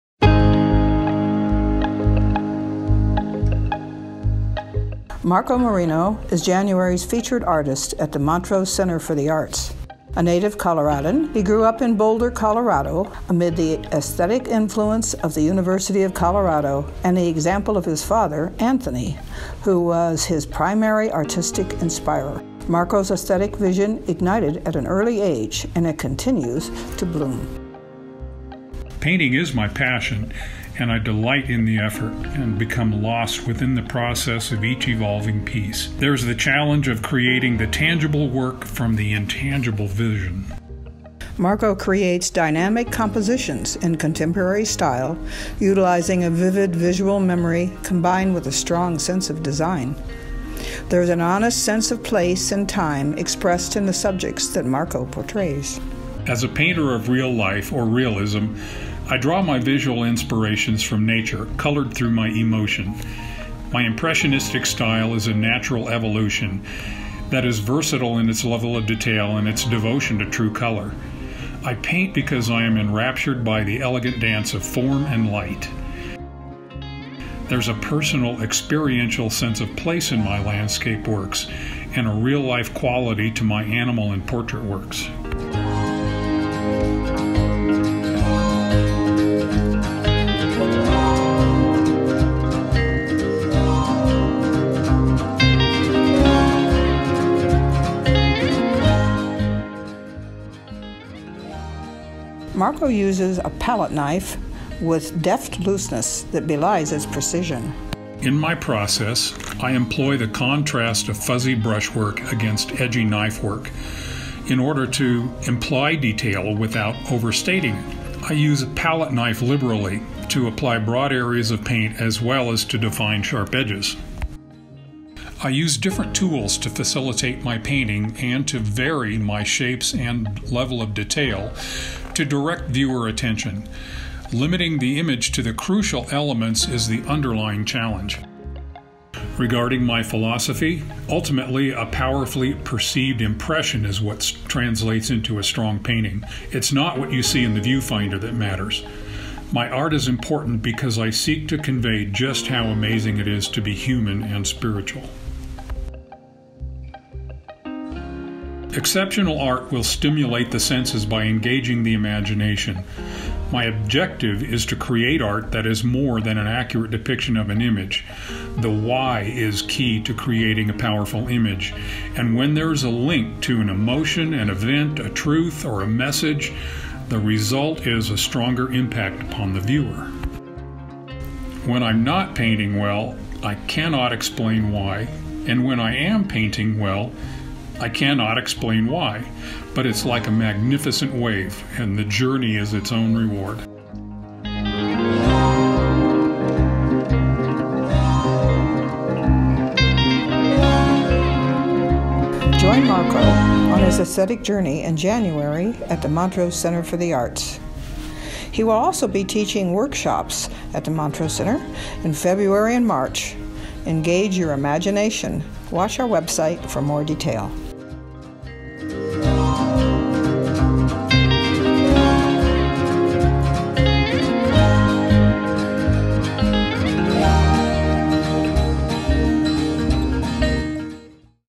The Art of Marko Marino
Coloradan artist, Marko Marino, reveals the particularities of his painting style, his painting philosophy, and his creative vision. Marko is MCA's January featured artist and our newest instructor! He's teaching classes and leading workshops in the new year.

Check out Marko's paintings on display all month at MCA, 11 S Park Ave., Montrose, Colorado.

Engage your creativity by signing up for Marko's painting classes

Follow MCA on Facebook to stay up to date on your favorite local creators: